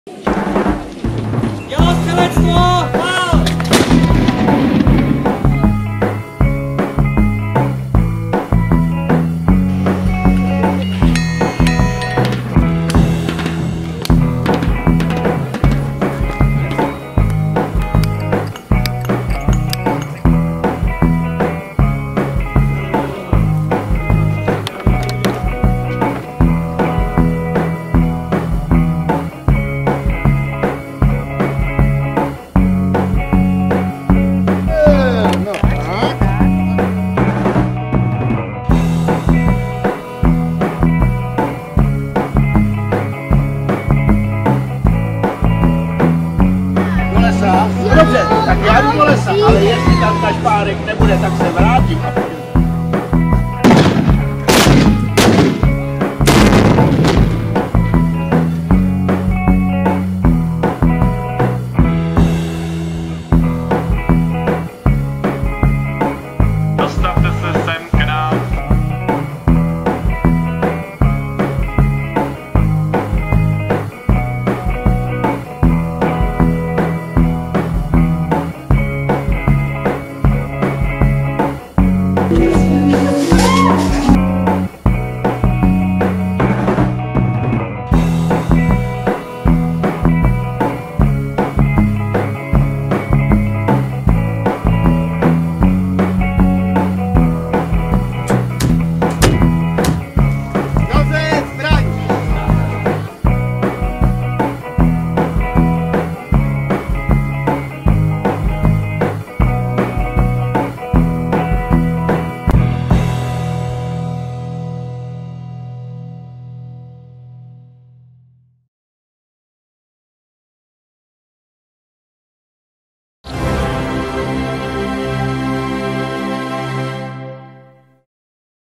Rok 2018, turistický areál Bastion I a podzemí v Josefově
V Josefově jsme společně prožili skvělý rok, nemyslíte? Přinášíme Vám krátké shrnutí toho, co jsme letos stihli. A to se zdaleka vše nevešlo.
Nechte se okouzlit atmosférou akcí Josefovský Masopust, Otevírání pevnosti Josefov, Josefovské Čarování aneb Oslava jara, Oživlý Josefov, Špitál ART nebo Štědrý den v pevnosti.